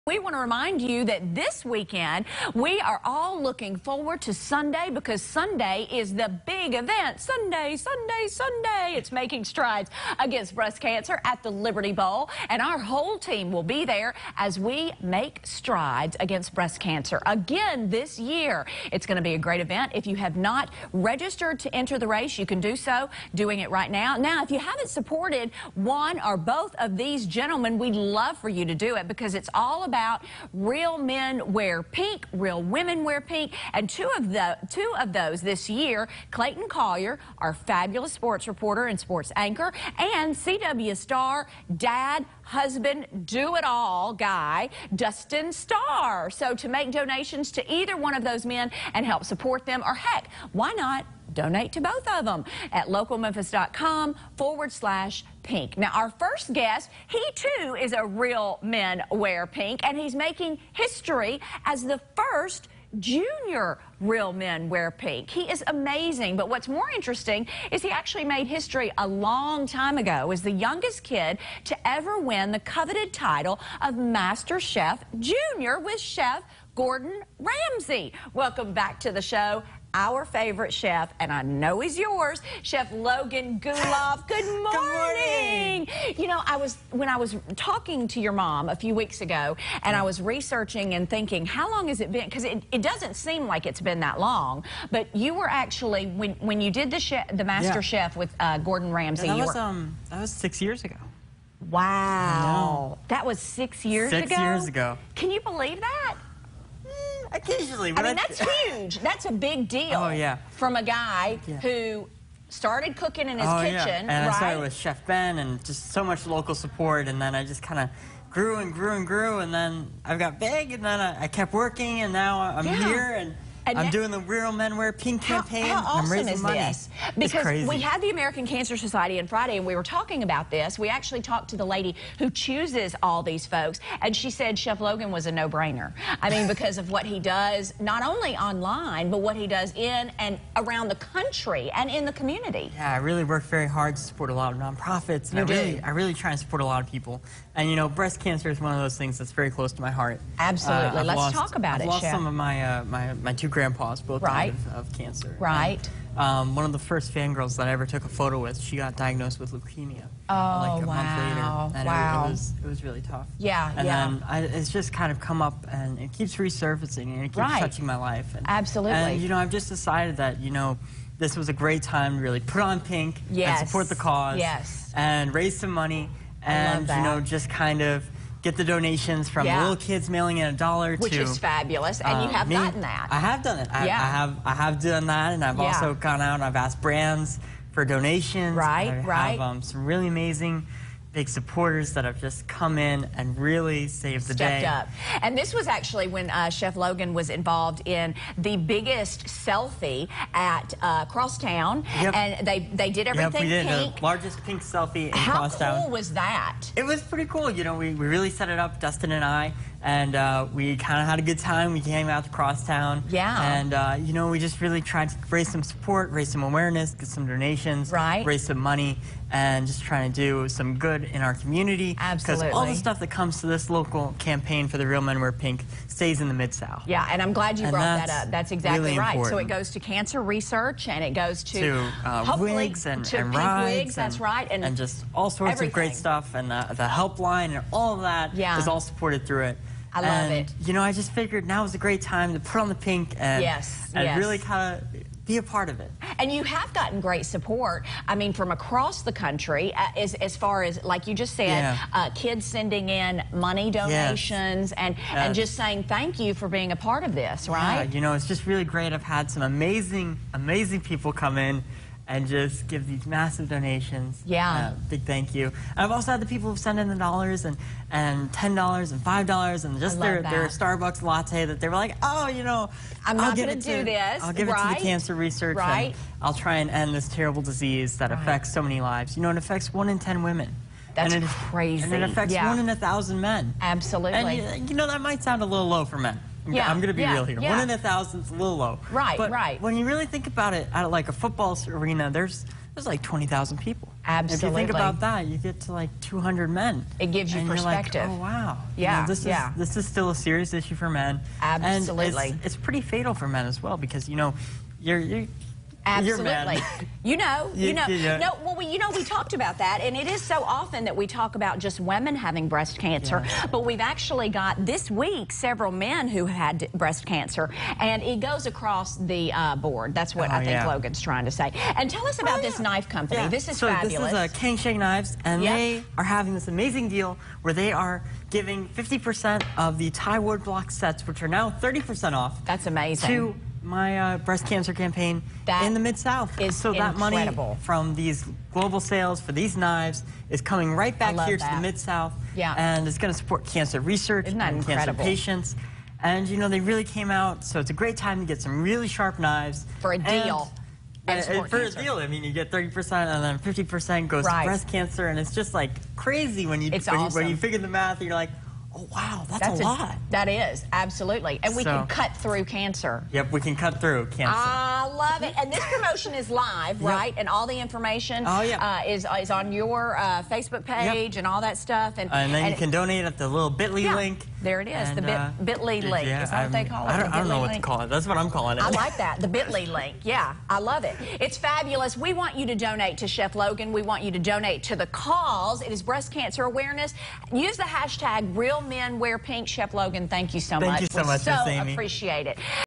Logan Guleff - Real Men Wear Pink Charity Cancer Fundraiser
Real Men Wear Pink Charity Fundraiser

American Cancer Society

Logan has been named one of the Most Influential Teens by Time Magazine and a James Beard Blended Burger Winner. Since becoming the 2014 Master chef Junior champion he has become a rising star in the culinary world. He was named Southern Living‘s Best New Southern Cook and earned a spot on Fortune Magazine’s 18 Under 18 list; he’s also the youngest certified judge for the World Championship Barbecue Cooking Contest in Memphis, Tennessee, and the youngest chef to cook at the historic James Beard House in New York City. Earlier this year, he judged the International Young Chef Olympiad in India and just launched his first cook, “Logan’s Chef Notes and Half Baked Tales”.